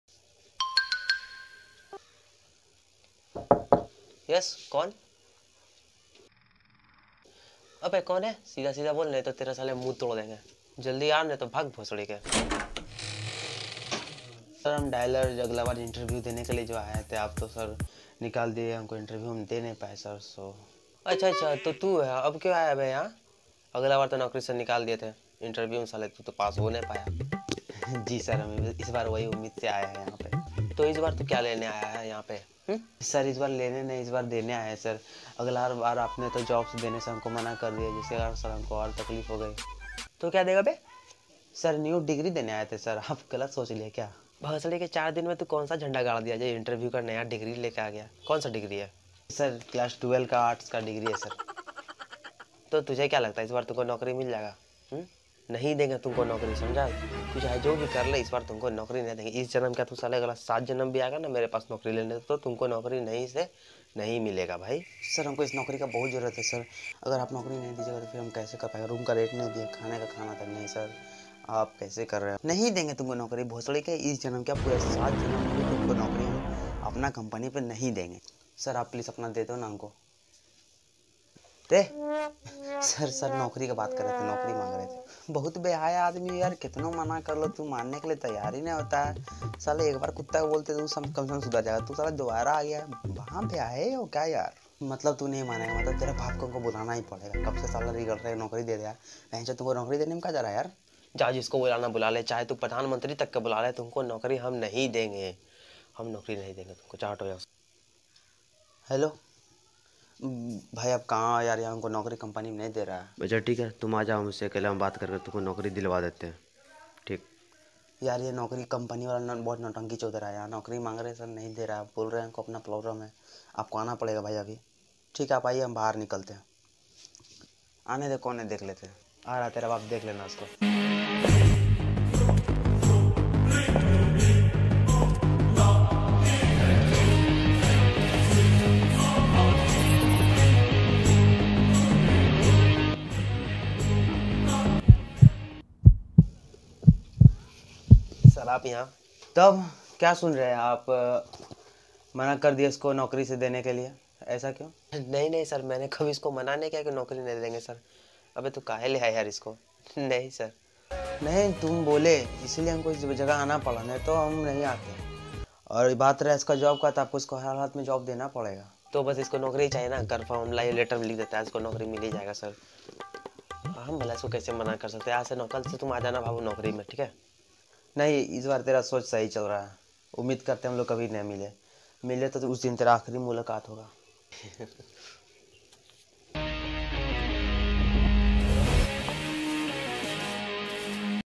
Interview part-2 | Narendra Vines. 👊🏻
In this Video My Favorite Actor
stylish Star ⭐ Allu Arjun sence
I Copy his Video sence

Hii Friends 💞
This My New Video 👍🏻
Plese like this

Aaj ki Video aapko pasand ayege
aacha lga to Support Kigea ga .